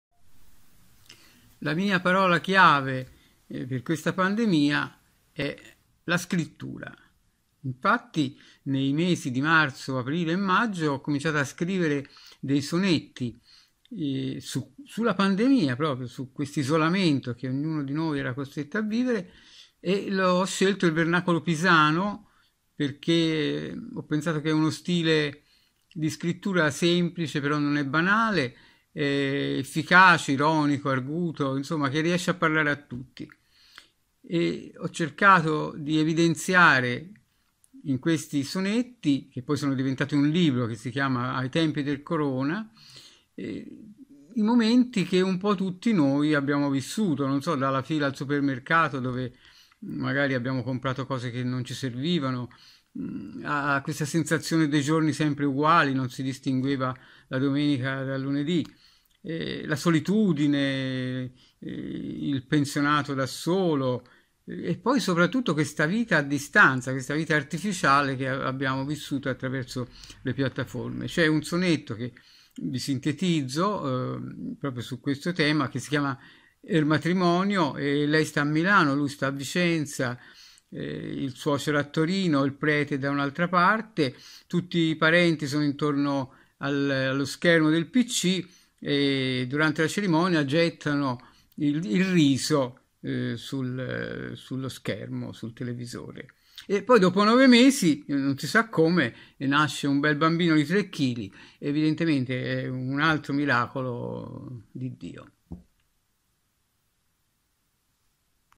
La poesia come cura per l'anima ferita dal Covid
E' scrivere la parola per guardare oltre l'emergenza sanitaria scelta dall'artista pisano Piero Nissim